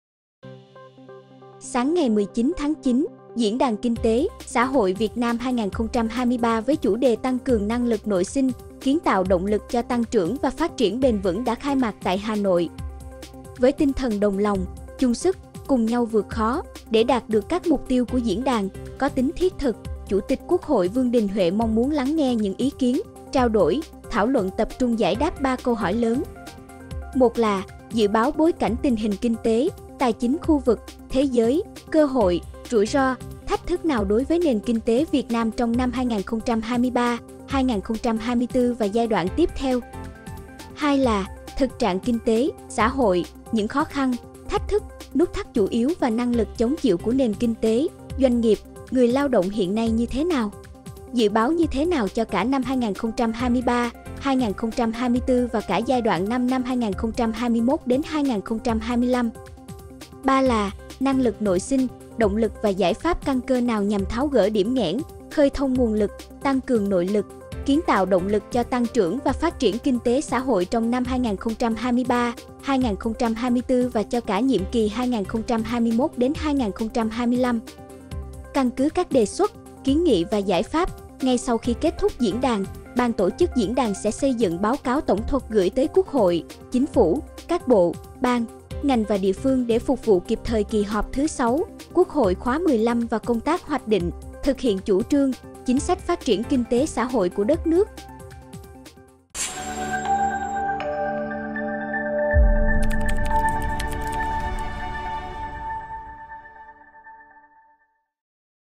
Chủ tịch Quốc hội: Mong muốn Diễn đàn tập trung giải đáp 3 câu hỏi lớn
Sáng ngày 19-9, Diễn đàn Kinh tế - Xã hội Việt Nam 2023 với chủ đề “Tăng cường năng lực nội sinh, kiến tạo động lực cho tăng trưởng và phát triển bền vững” đã khai mạc tại Hà Nội.

Với tinh thần “đồng lòng, chung sức, cùng nhau vượt khó”, để đạt được các mục tiêu của Diễn đàn, có tính thiết thực, Chủ tịch Quốc hội Chủ tịch Quốc hội Vương Đình Huệ mong muốn lắng nghe những ý kiến, trao đổi, thảo luận tập trung giải đáp 3 câu hỏi lớn.